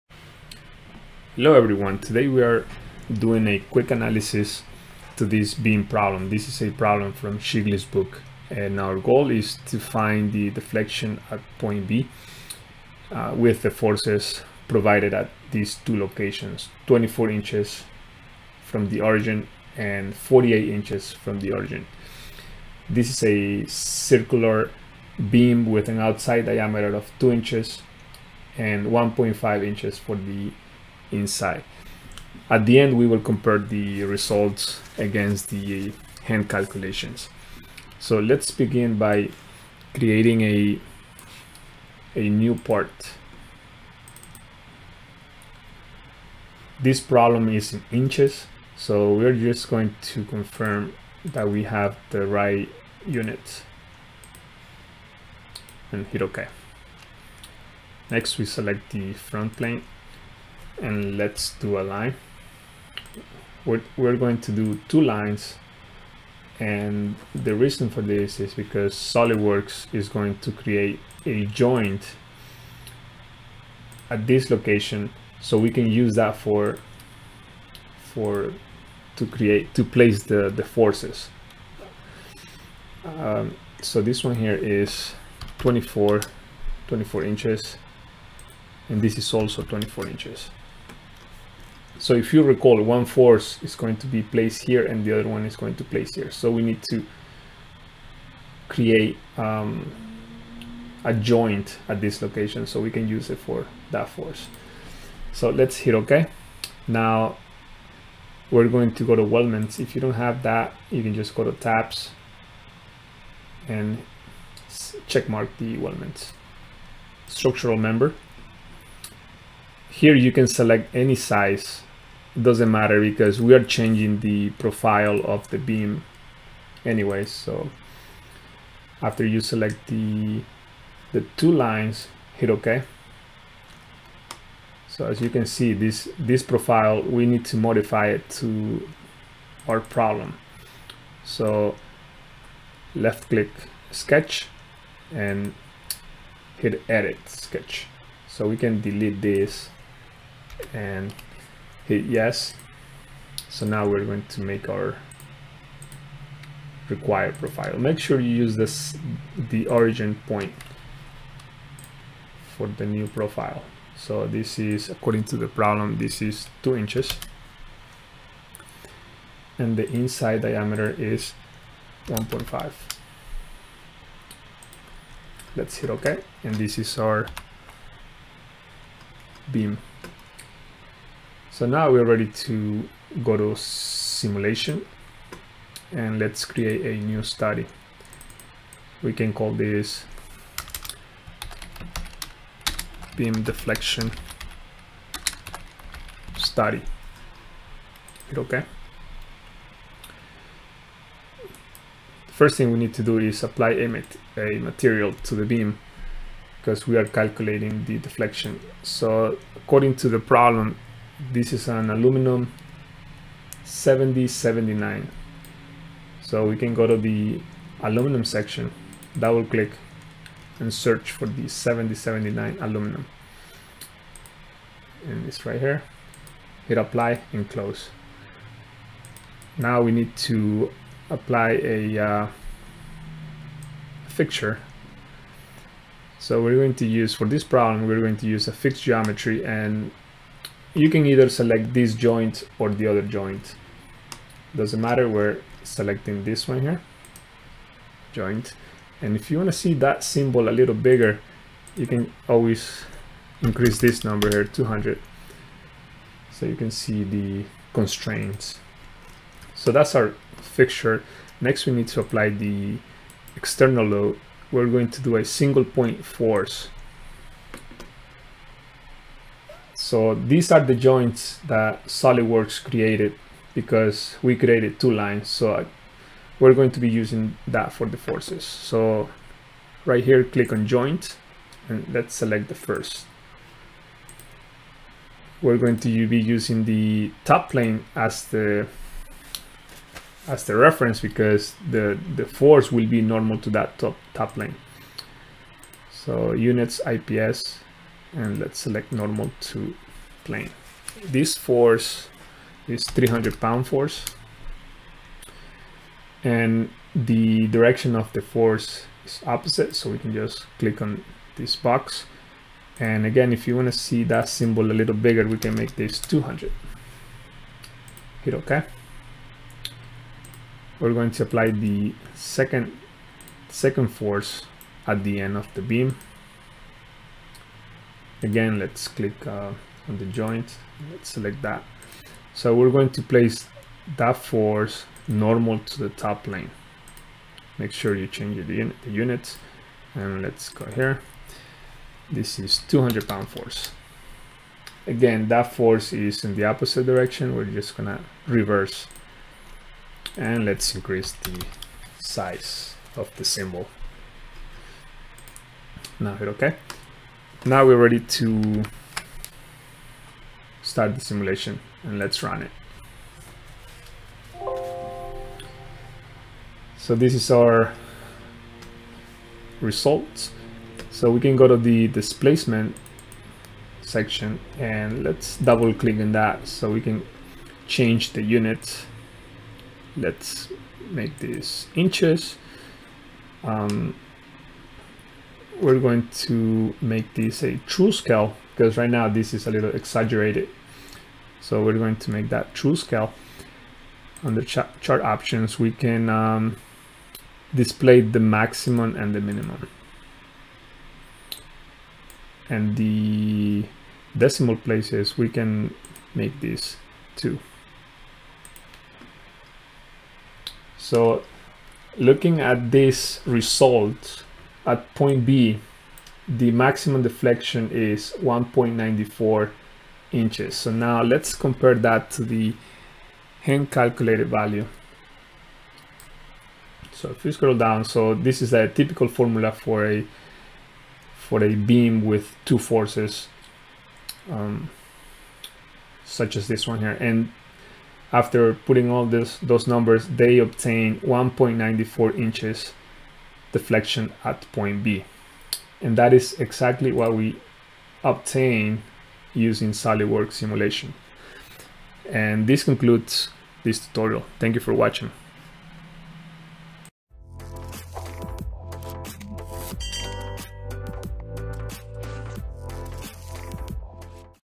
Cantilever Beam Deflection | SolidWorks Simulation for Beginners | FEA Analysis #2
On this video tutorial we are going to learn how to set up a circular beam profile and calculate the maximum deflection at the end of the beam. This is a typical classroom problem, you can find this problem on Shigley's design book. SolidWorks Result are then compared to the hand calculated values.

Learning Outcomes from this video:
• Setup circular beam for simulation
• FEA (Finite Element Analysis)
• Beam Deflection of Cantilever Beam
• Read and modify results
• Use of Weldment tool